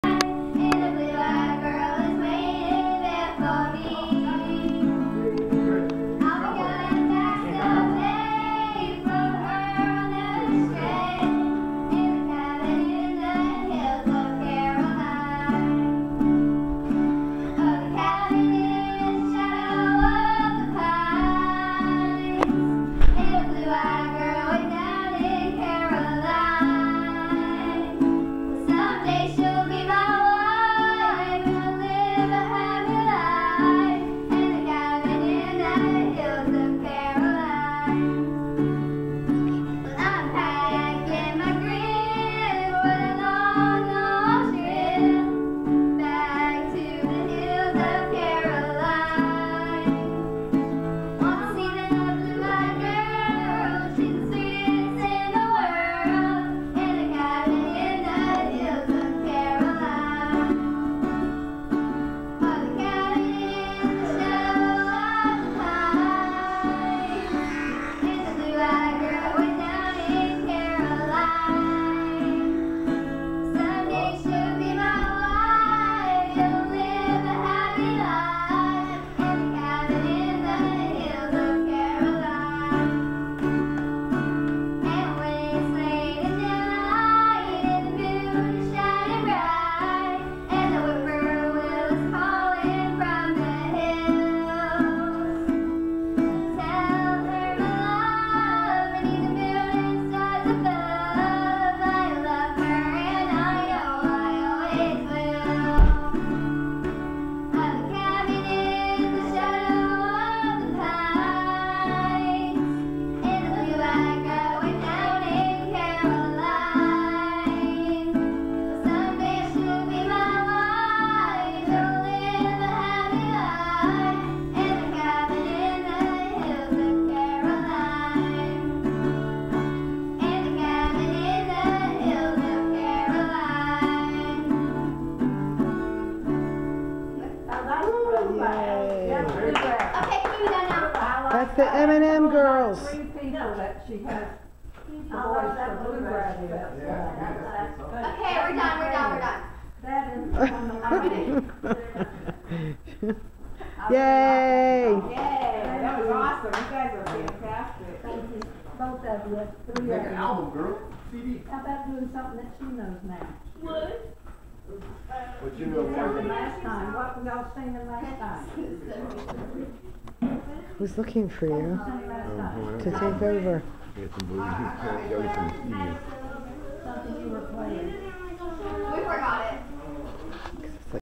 The M&M Girls
Makayla and Madison singing bluegrass for Meme.